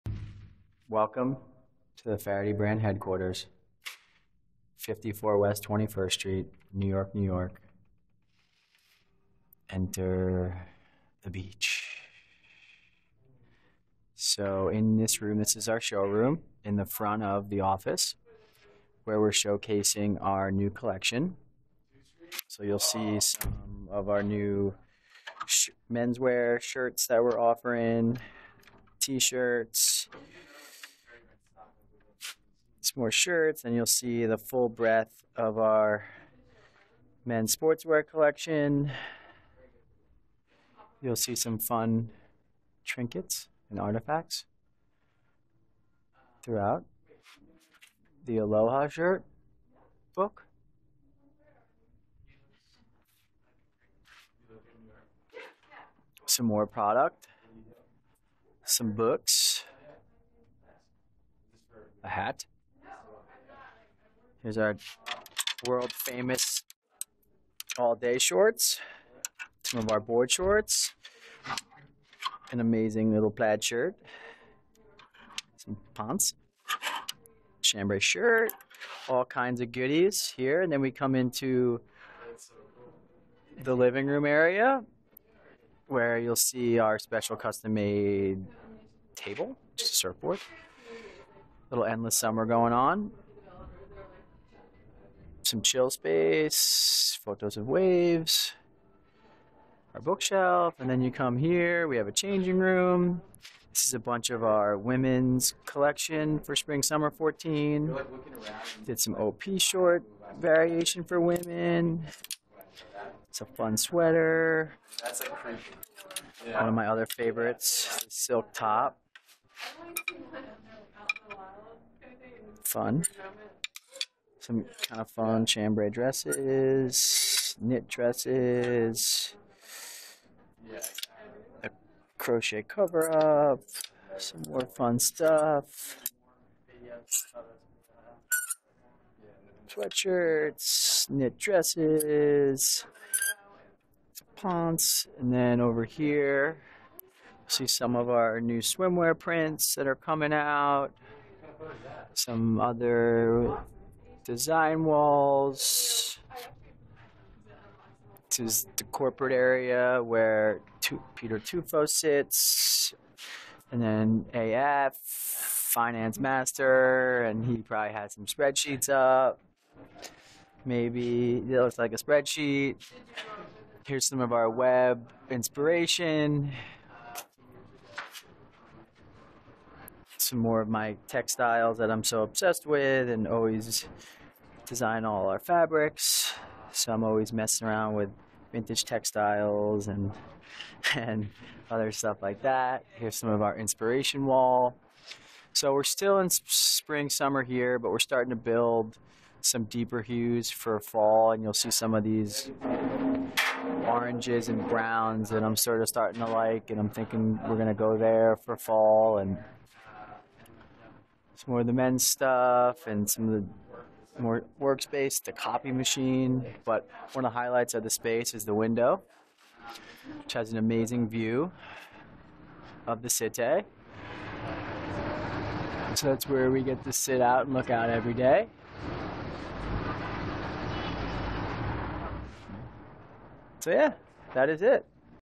Faherty Brand - Guest of Glass - Google Glass
Mike and Alex, the twins and founders of Faherty Brand, show you around their NYC showroom while wearing Google Glass. Menswear, shirts, swimwear, sportswear and more! Their collections are beach inspired with eco-friendly construction - and oh so fun!